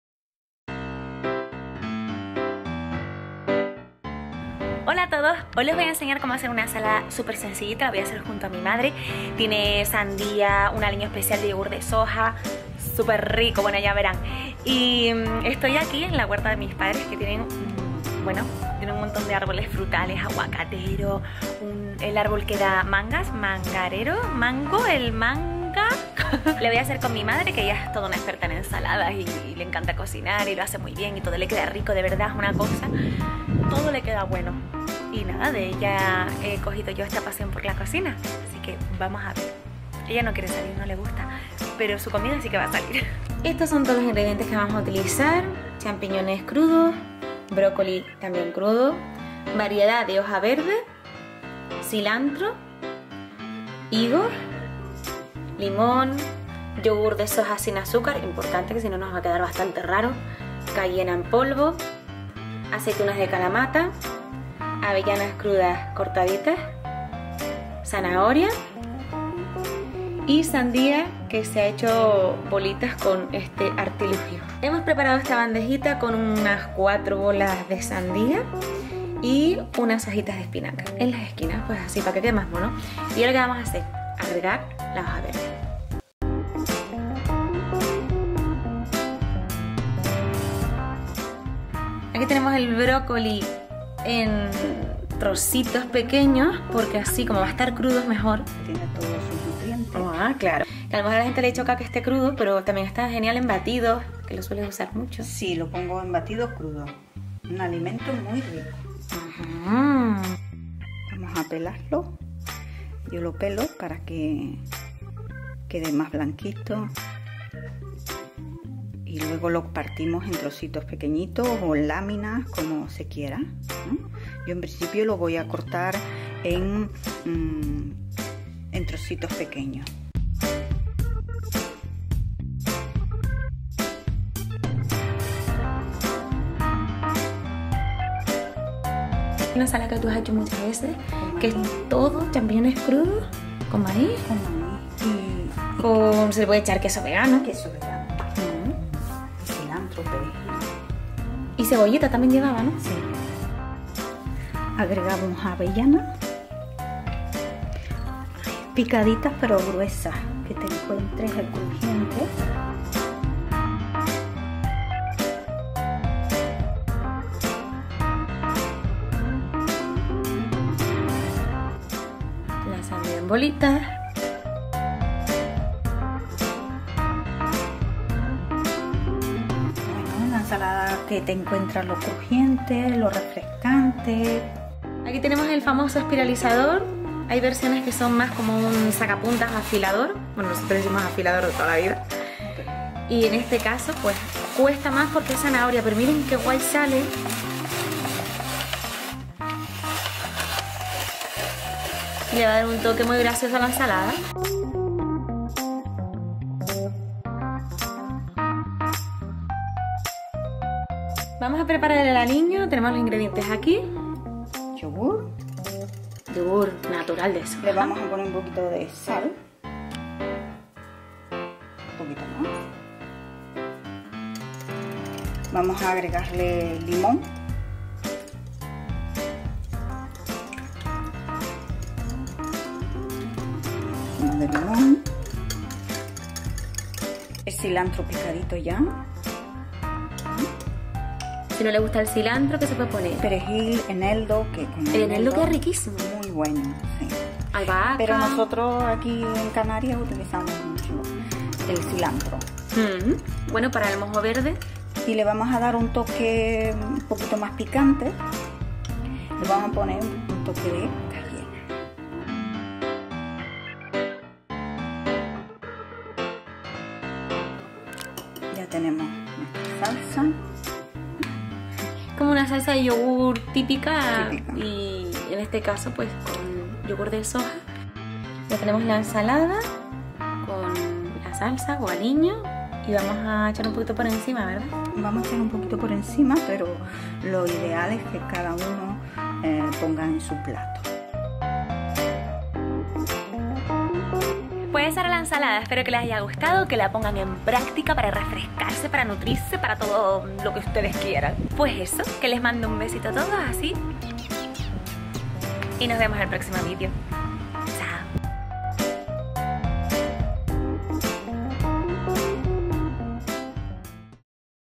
Ensalada veraniega con aliño bajo en grasas
¡Hola a todos! Echen un vistazo aquí abajo ;)

Mi madre y yo les presentamos una ensalada refrescante y nutritiva  con un aliño muy ligero ¡está verdaderamente rica!

¡Espero que les guste y que les sea útil!